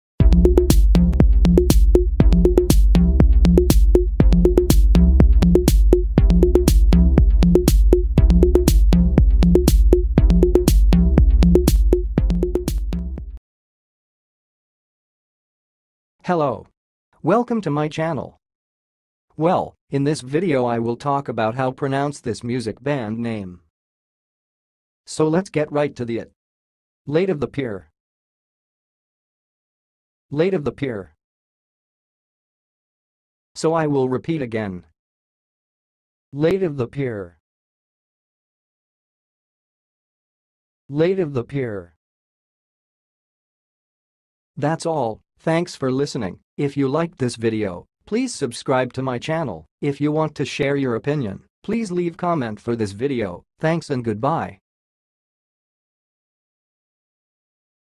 LATE OF THE PIER - HOW TO PRONOUNCE IT!? (HIGH QUALITY VOICE)
Watch and listen how to say and pronounce Late of the Pier music band name!
Compare your pronunciation with our video!